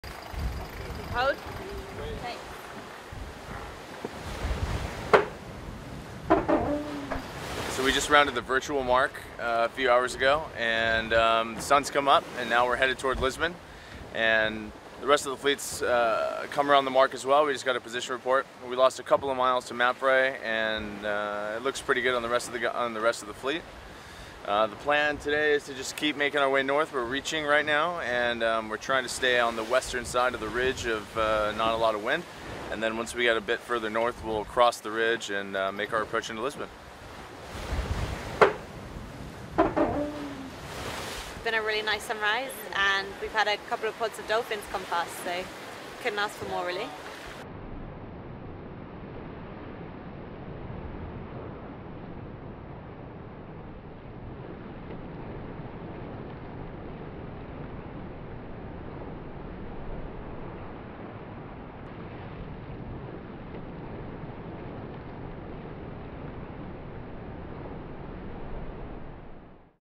Boat Log - Leg 1: Day 5 - Hannah Diamond
The team holds the lead around  the virtual mark.

"Its been a really nice sunrise, and a couple pods of dolphins have come past. Couldn't ask for more really." - Hannah Diamond